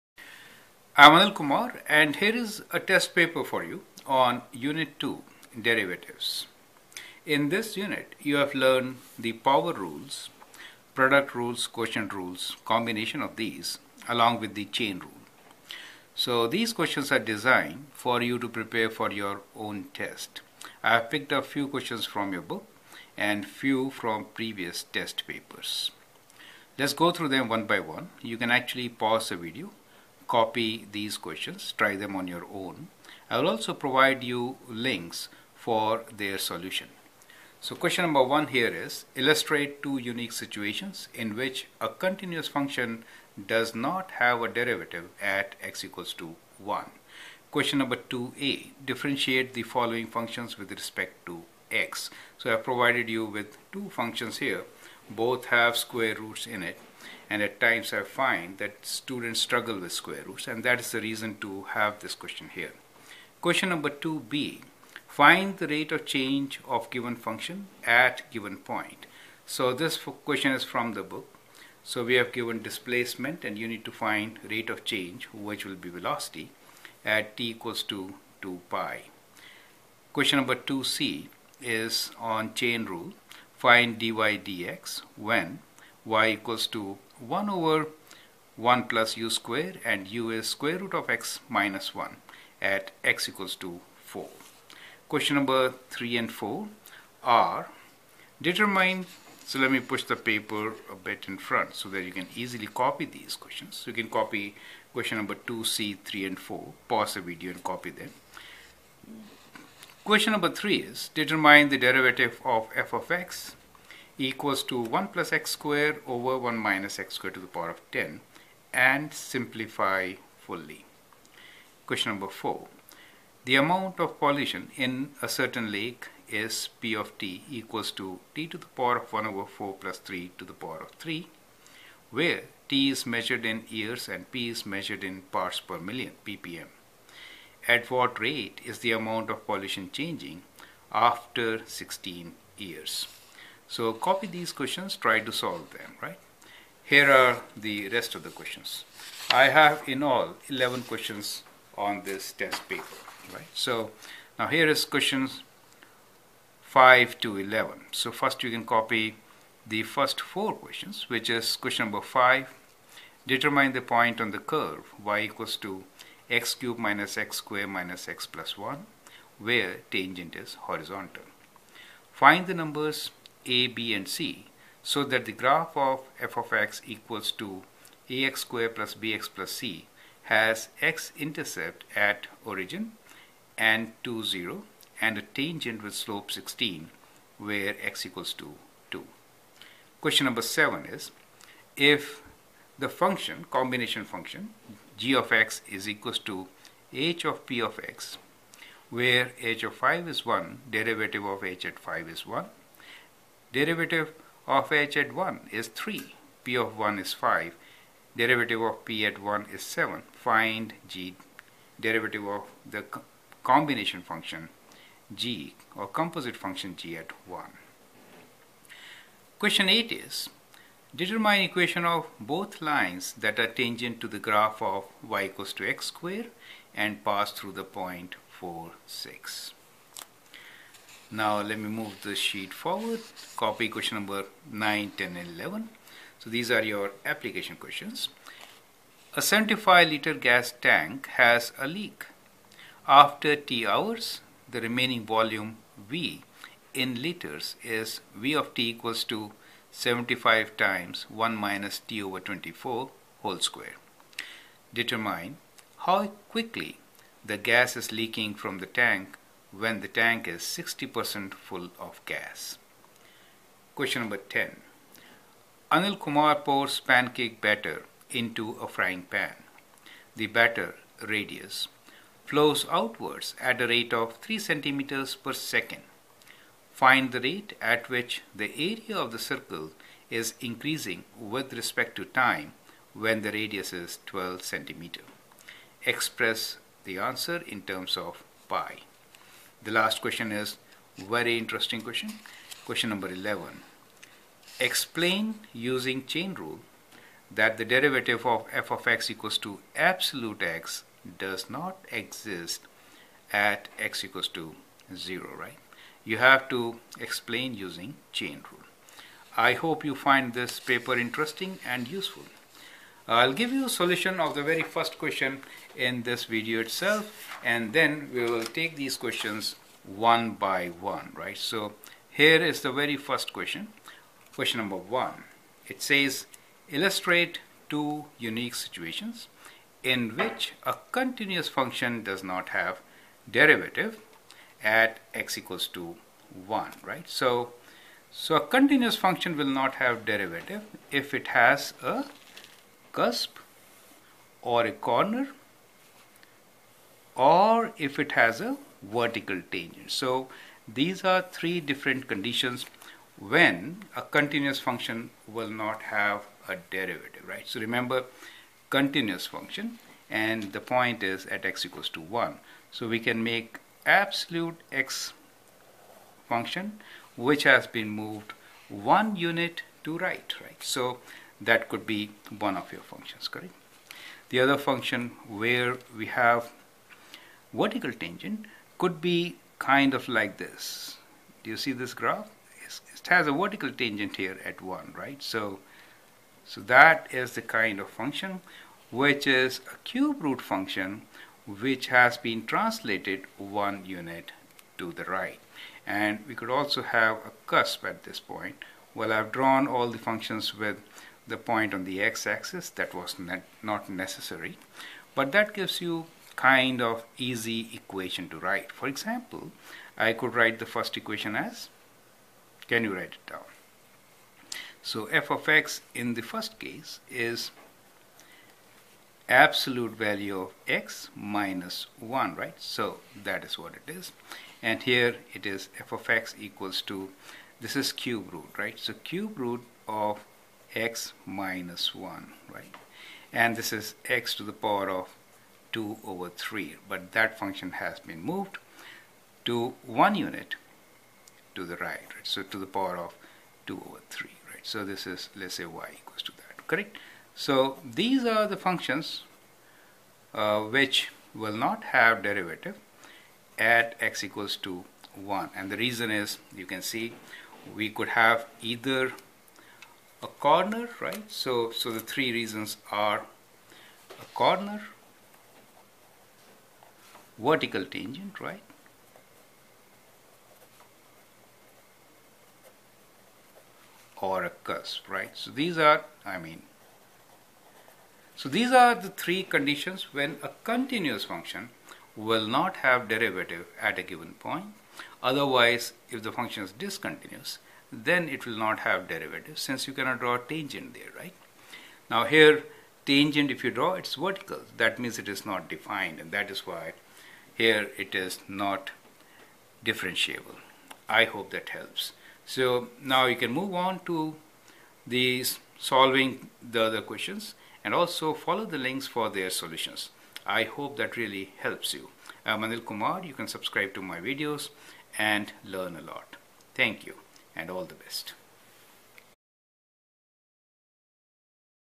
Unit 2 TEST Derivative Chain Rules Applications Test MCV4U Calculus
All the Best for Grade 12 Calculus Students. Here is the Test Paper to Practice !